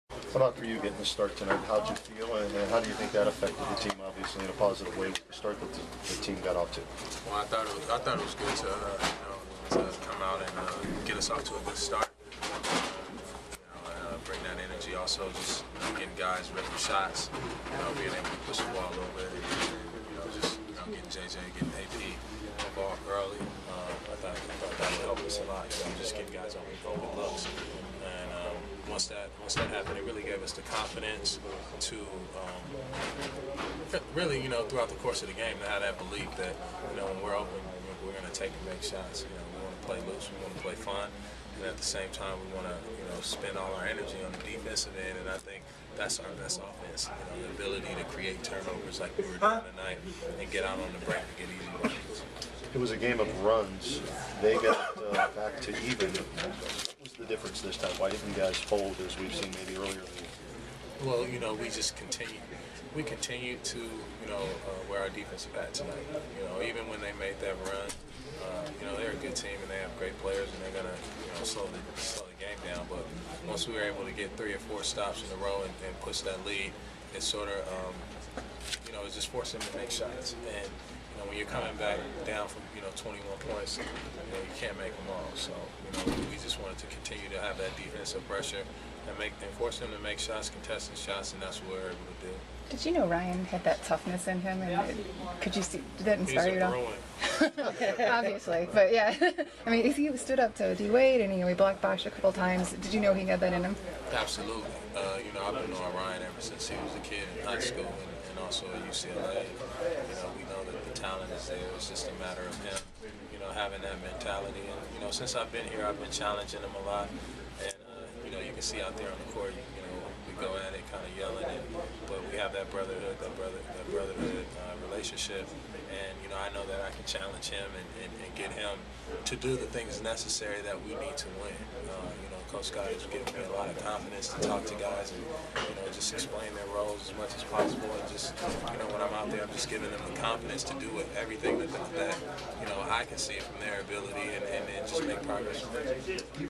Stepien Rules: Baron Davis after Cavs beat Heat
Baron Davis postgame after Cavs beat Miami Heat